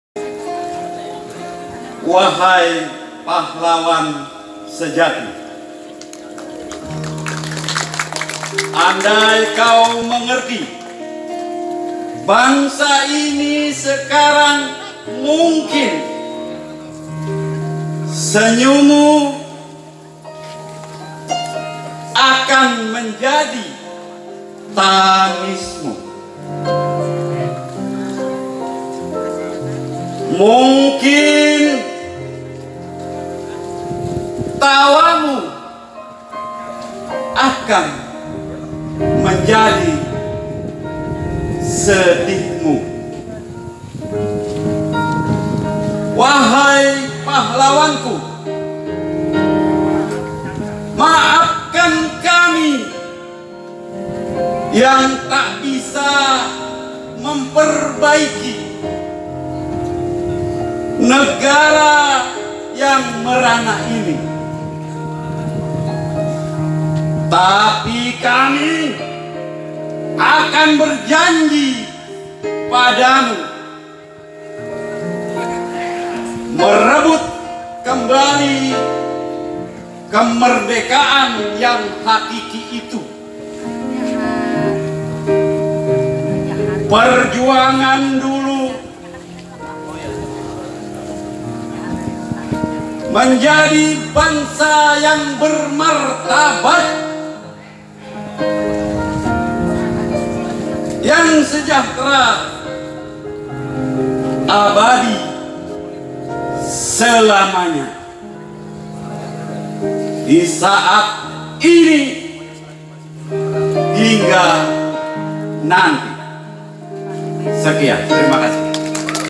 PUISI PERJUANGAN Oleh Kepala Lapas Palangka Raya
Pembacaan Puisi oleh  Kepala Lapas Palangka Raya, Bapak Syarif Hidayat di acara The Gathering of Heroes dalam Rangka Memperingati Hari Pahlawan tahun 2018